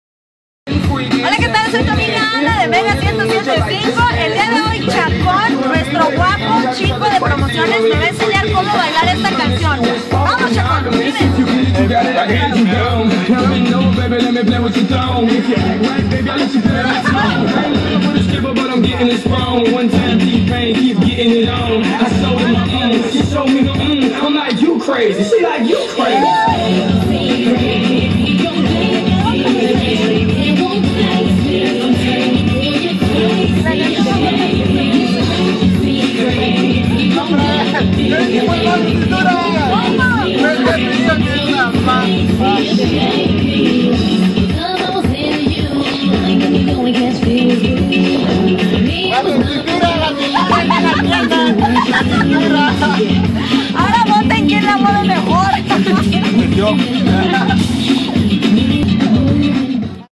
Chacon Ensena A Bailar A Ana
Asi nos divertimos en las promociones de Mega 107.5, Chacon uno de nuestros chicos de promociones, me enseño como bailar, creo que para nada compito con el jajajaja